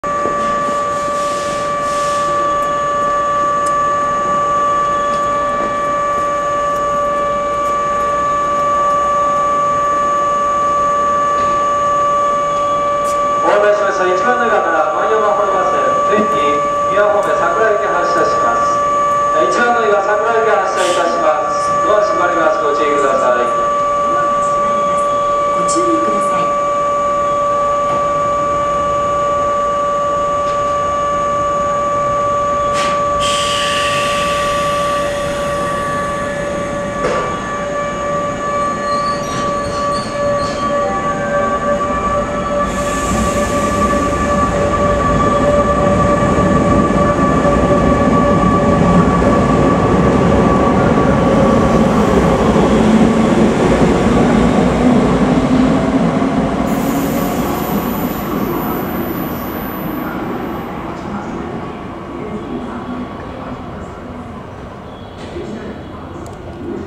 ＪＲ桜井線105系４扉車出発＠奈良
ＪＲ桜井線や和歌山線で主に運用されている４扉の103系。
奈良から和歌山まで通し運用もされています。
撮影は奈良駅にて。